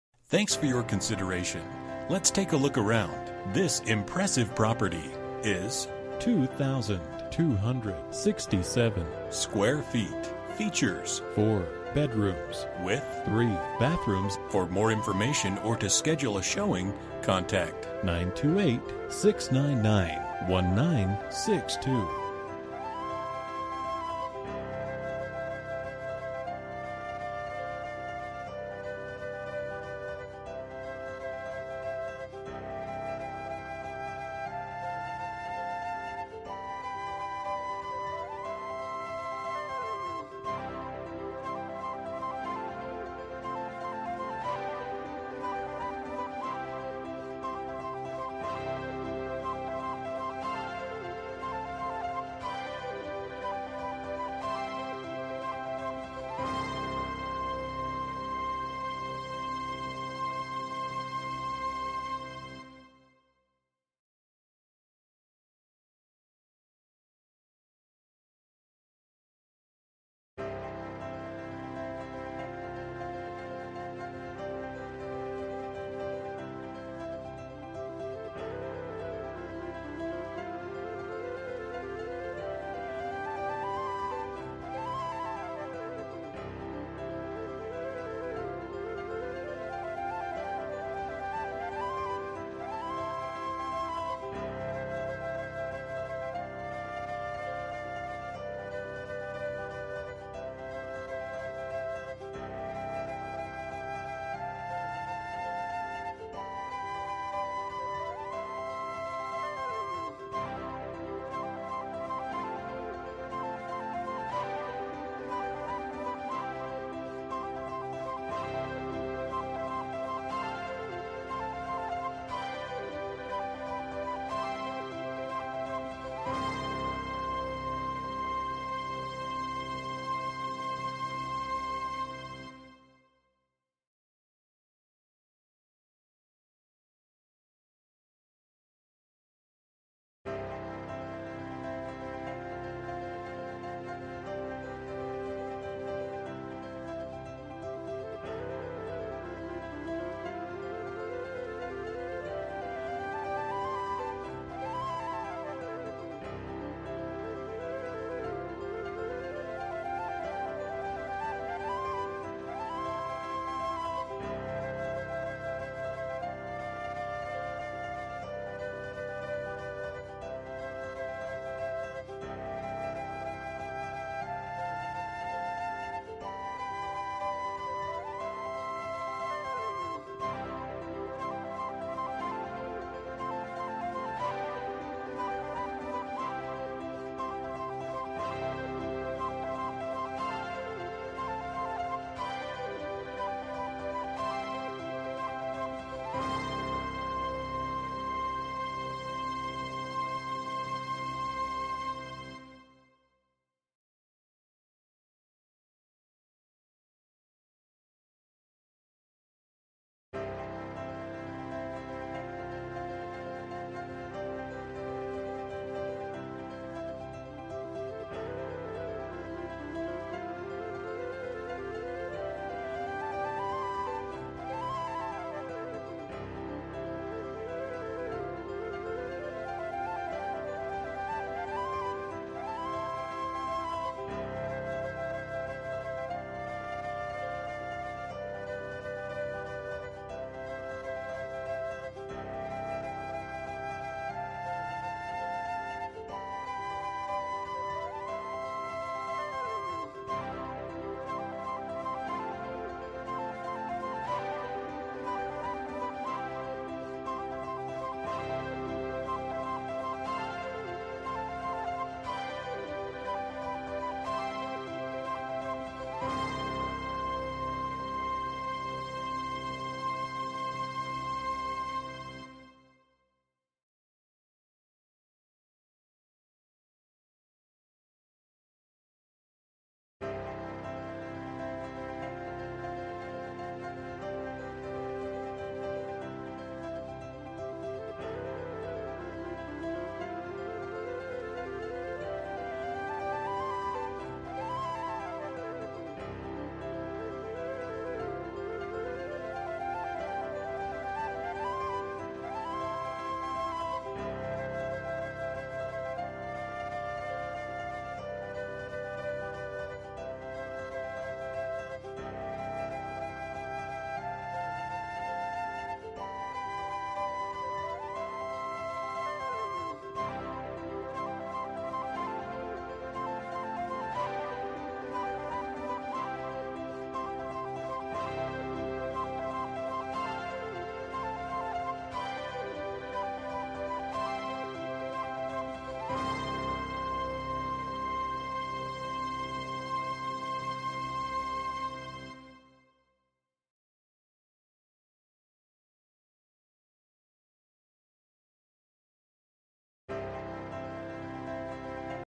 Home for sale - 1100 223RD Lane, Buckeye, AZ 85326
This IS the home for you - if you're looking for a birdwatcher's paradise while you sip your favorite beverage in the backyard that borders the golf course - you don't need to look any further. While in the backyard you will enjoy a lighted walkway, a fully installed sprinkler system and large shade trees along with multiple seating areas. Upon entering this enchanting home you'll be be wowed by the decorator paint, newer flooring, updated kitchen and bathroom (including ''subway'' tiled full shower on the main level), new cabinetry, in the laundry room, which includes the washer and dryer, and direct access to the garage. The newly painted upstairs offers the spacious Master Bedroom, 3 additional bedrooms, loft and family room. All this and minutes from shopping and Skyline Regional Park

Bedrooms: 4
Bathrooms: 3
Square Feet: 2267
Price: $255,000